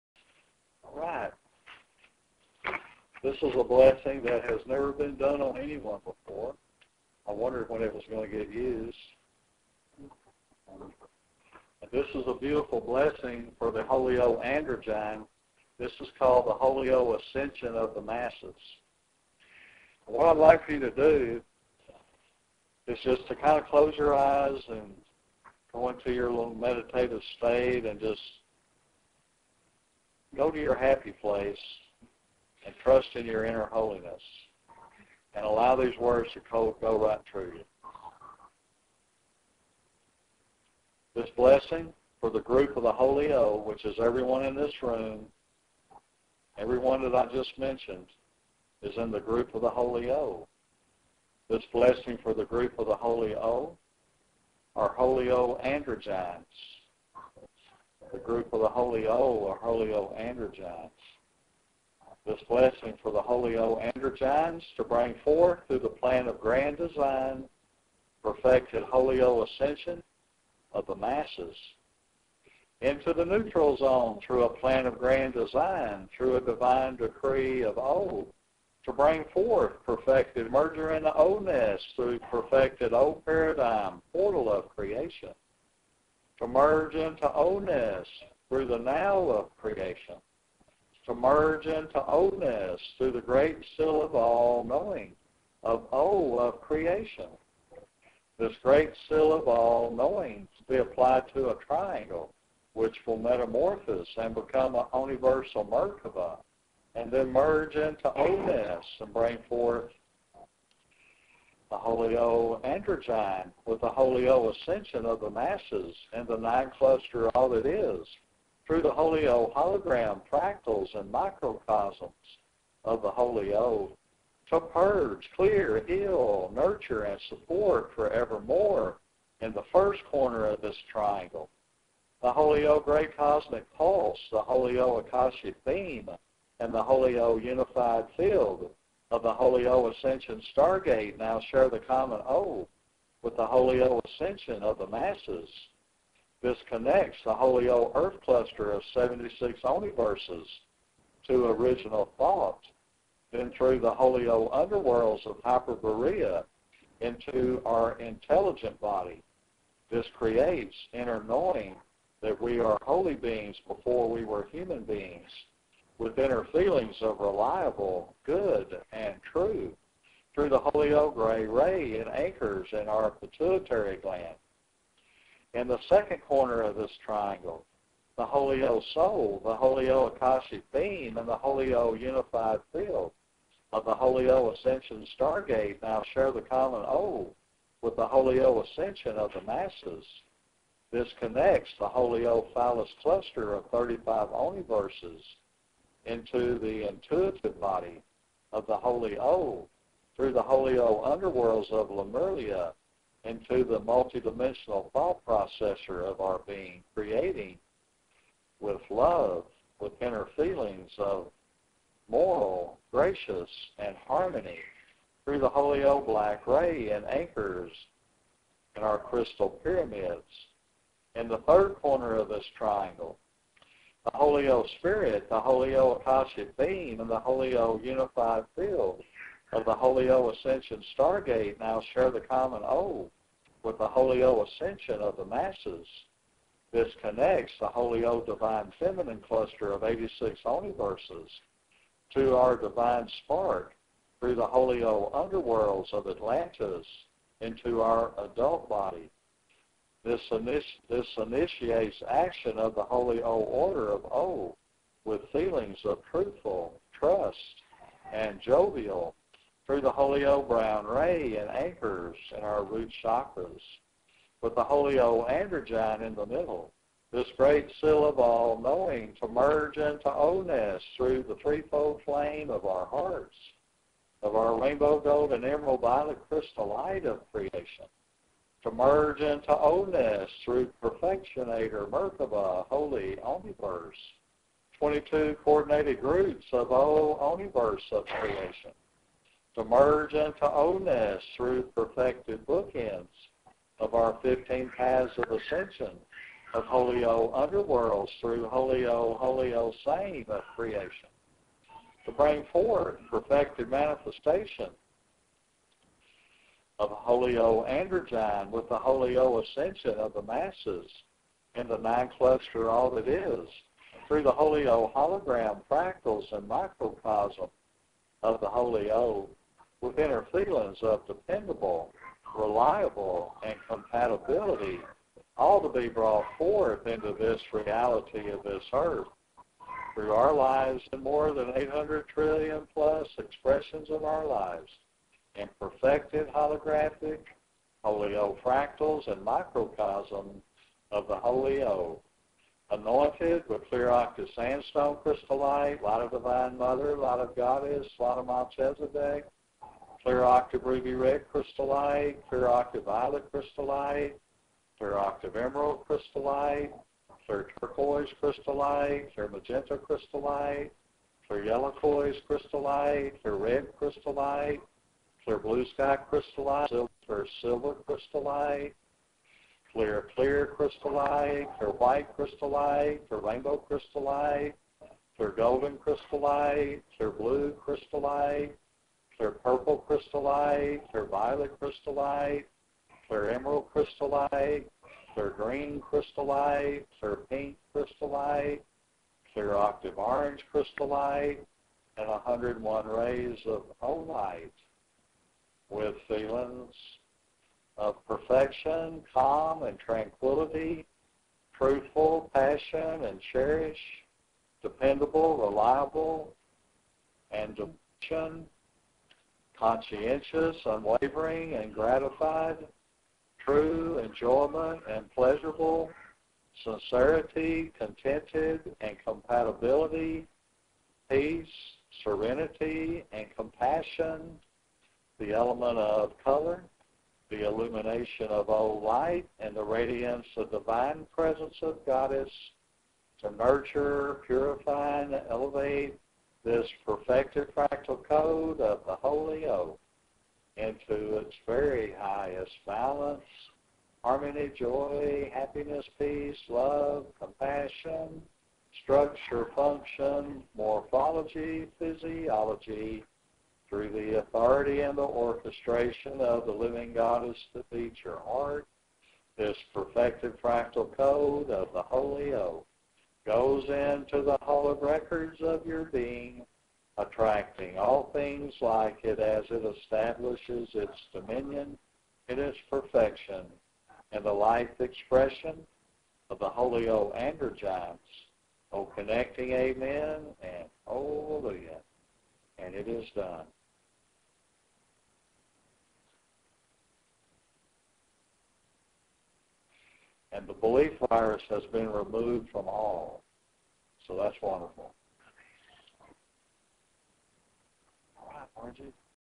Dr. Dan Mathews Holy Divine Healing
This is a blessing that came at the end of Dr. Dan's Murfreesboro TN Lecture on August 11,2014. This blessing had never been done before on anyone until Aug 11.